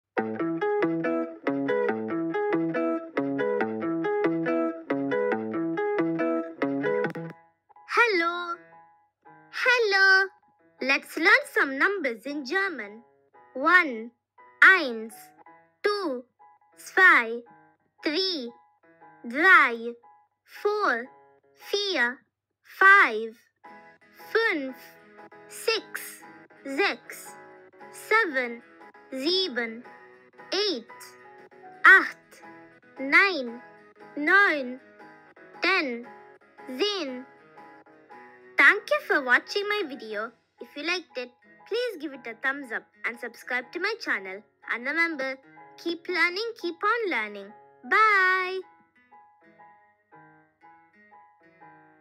Lets learn some Numbers in German Learning Languages by Languages With Nila
Hello my dear friends.Today, We are going to learn some Numbers in German with Hannah and Ella .Let's get started!